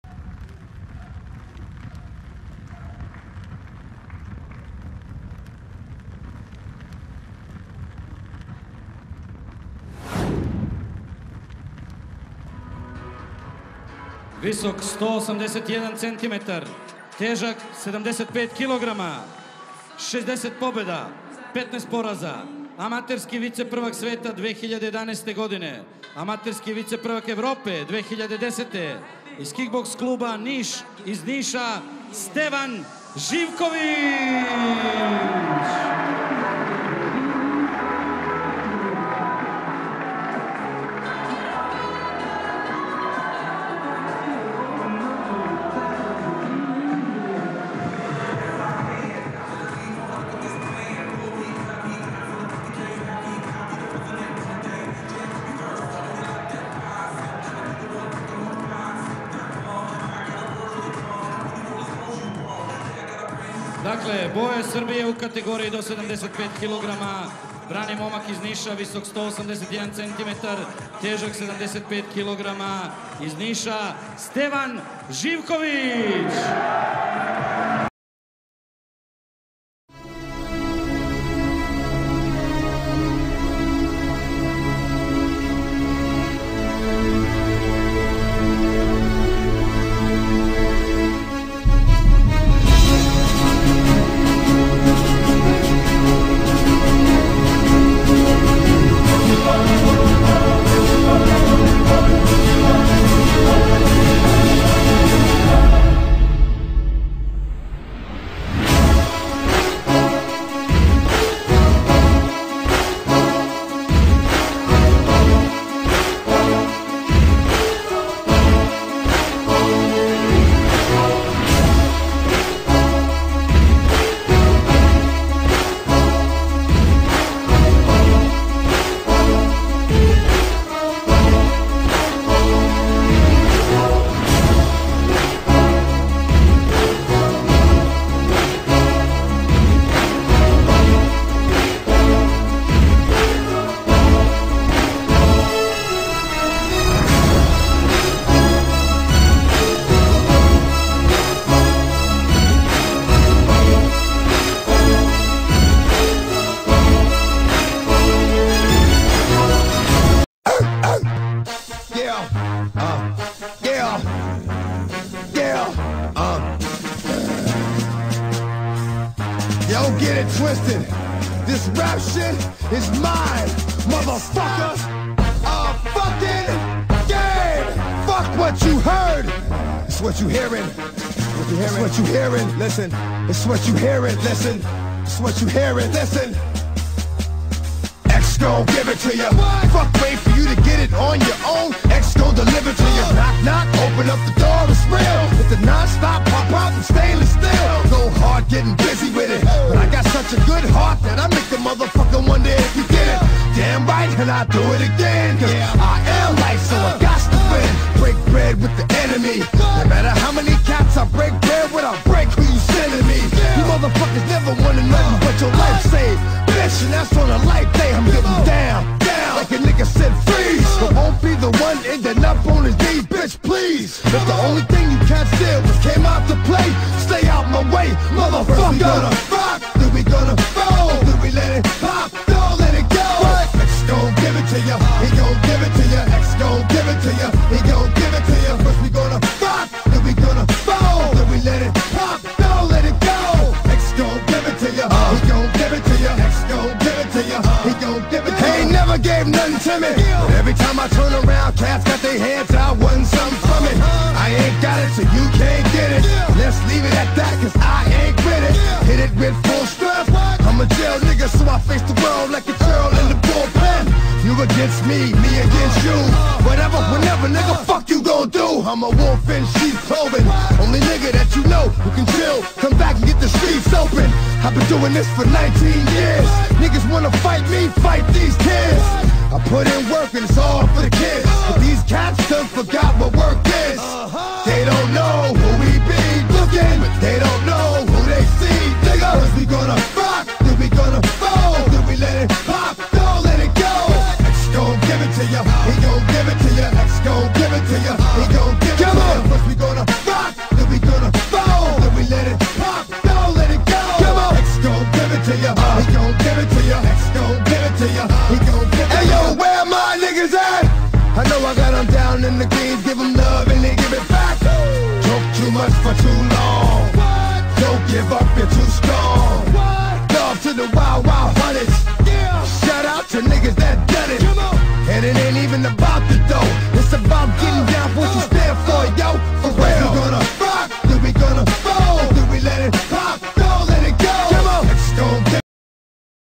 Stevan Zivkovic Highlights (Nokauti)
Knockouts and highlights from fights of Stavan Zivkovic